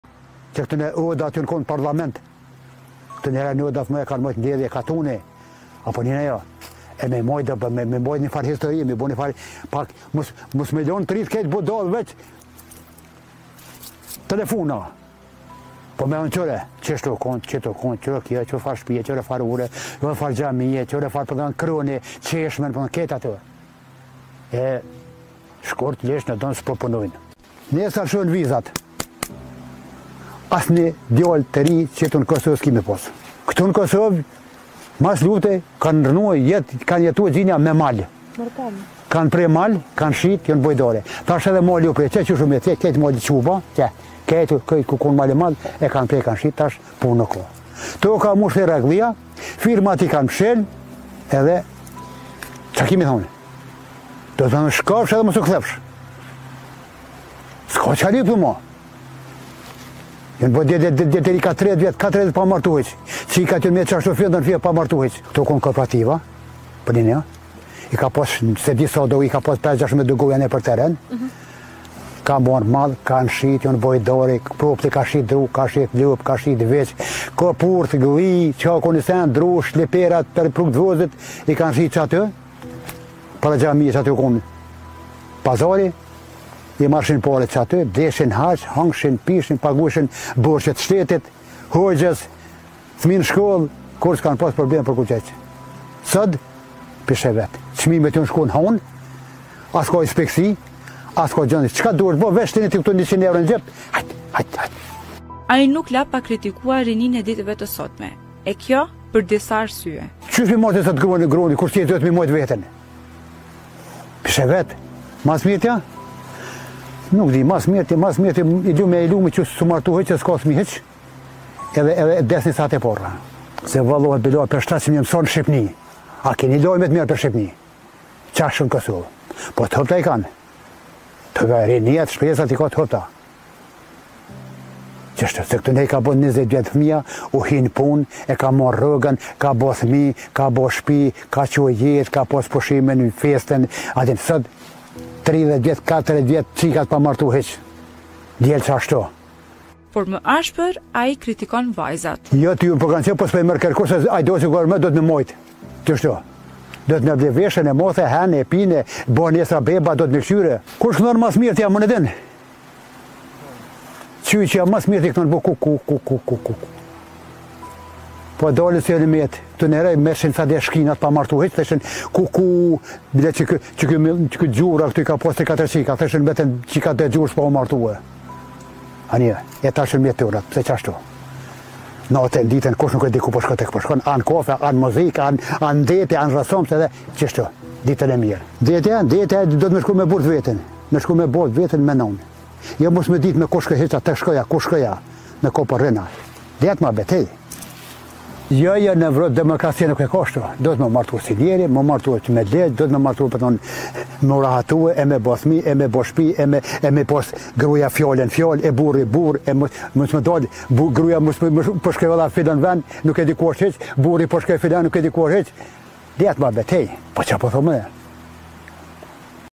"Te rinjte krejt budalle vec ne telefona" - Gani Haliti - Tregime nga katuni
"Me msitë se kurr se kom pa deri qatë ditë që u ardhë me kerrë të kuq" - Gani Haliti tregon se si u martua me msitë në moshen 18 vjeçare, ai tregon se asnjëherë nuk kanë pasur probleme si çift
"Marre çka po veshin femnat tona e po dalin në rrugë , a të vjen marre prej nane e prej babe" - Gani Haliti kritikon vajzat